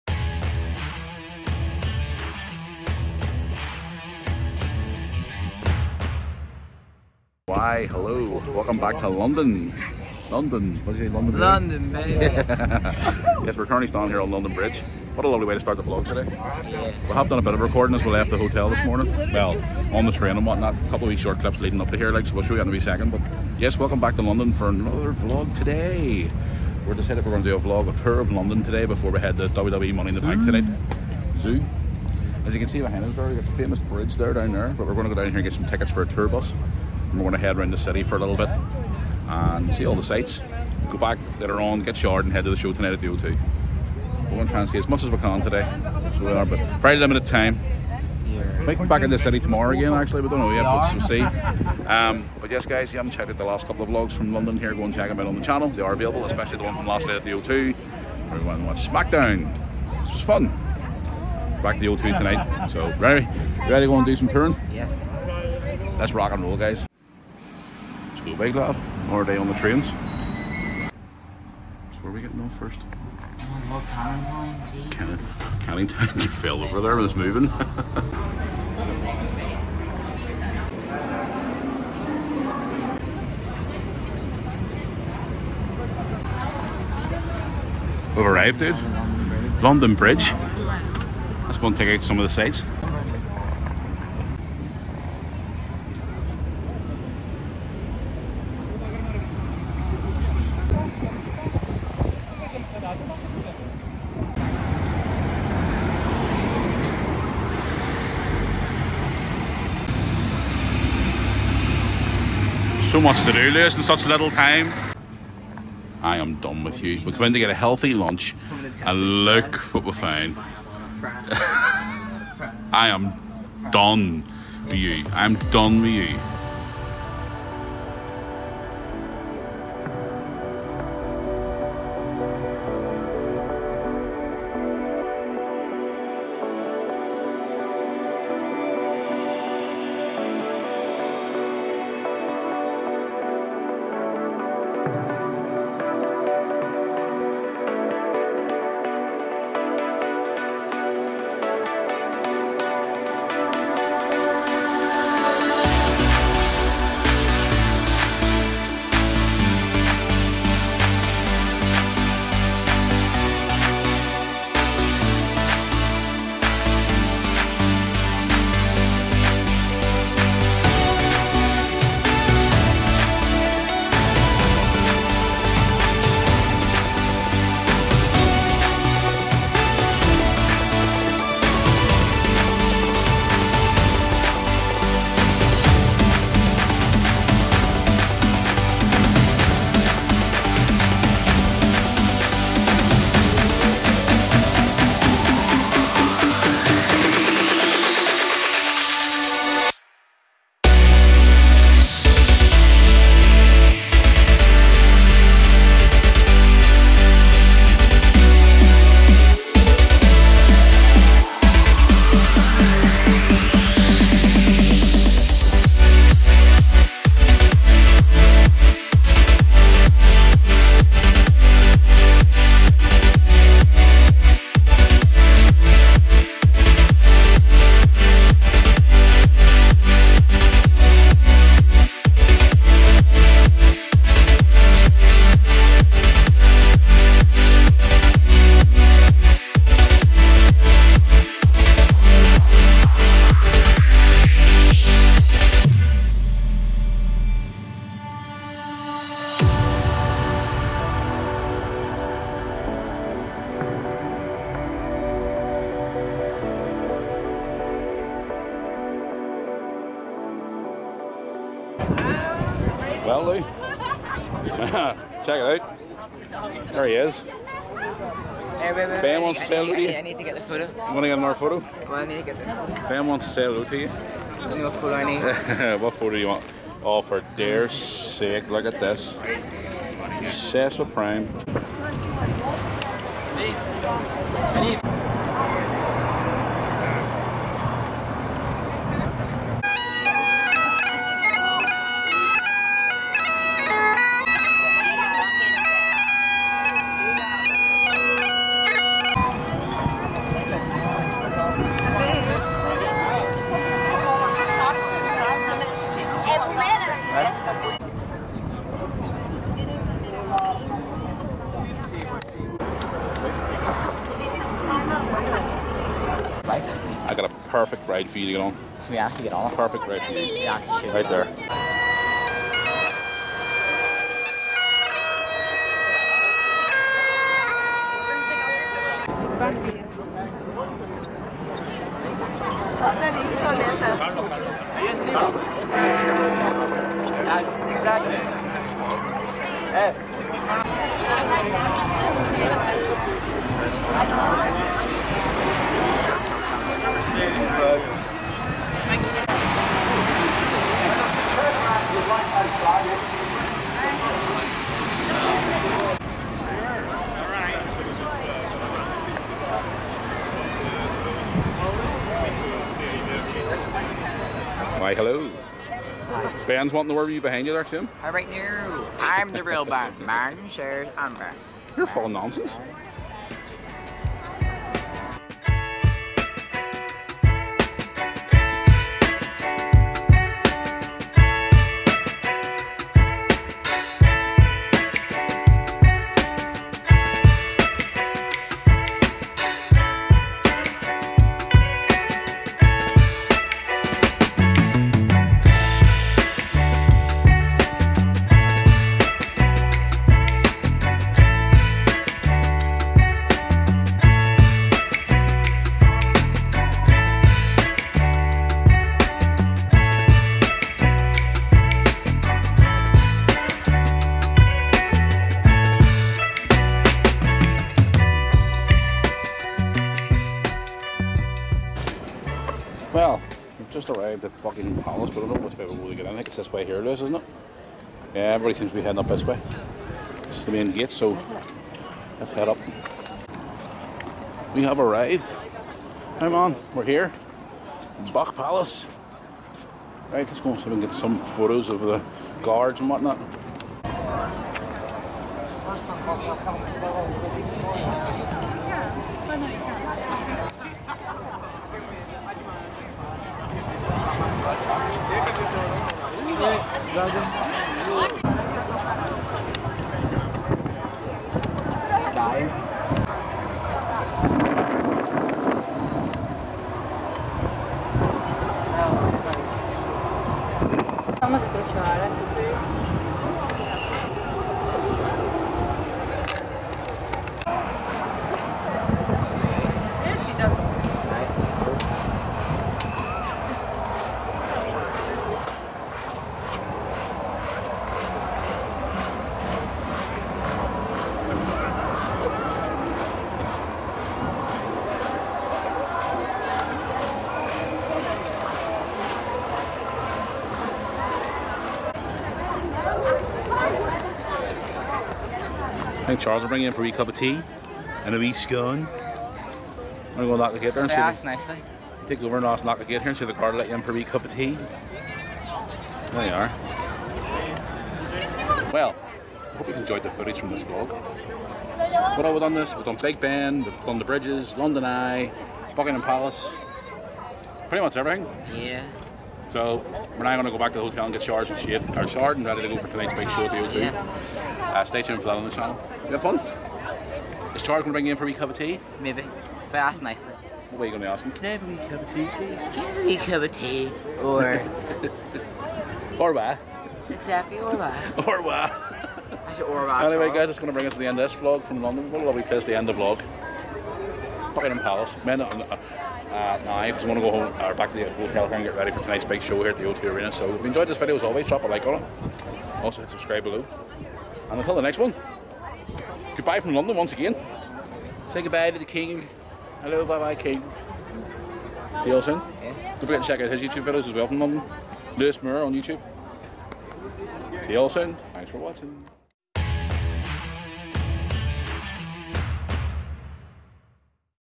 TOUR OF LONDON.
In this Vlog Matthew & Luis do a Tour around London.